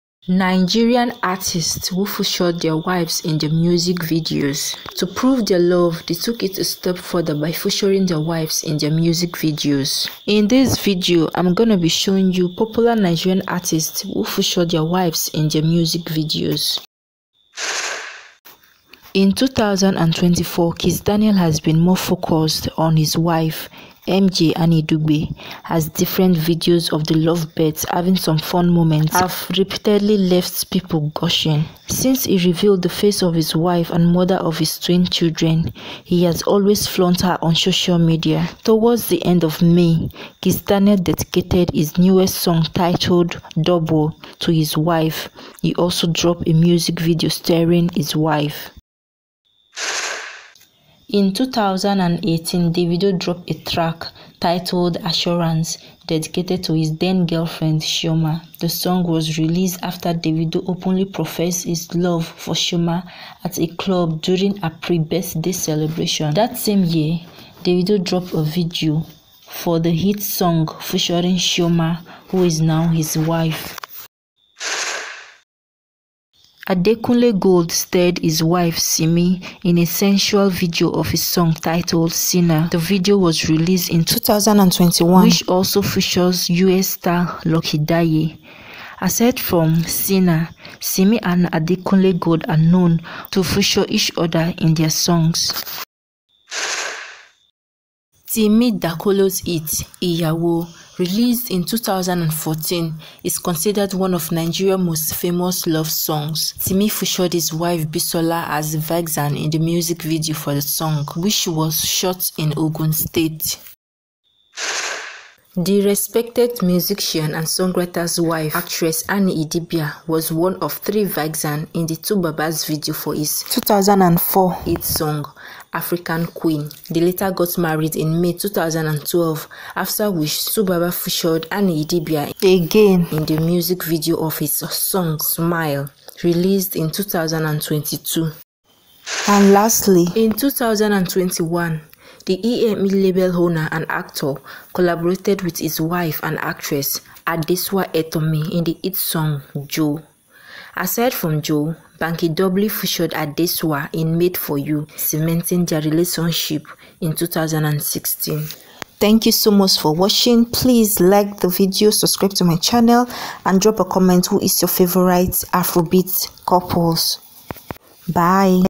Nigerian artist who featured their wife's in their music videos
In this video I revealed 6 Nigerian artist who featured their wife's in their music videos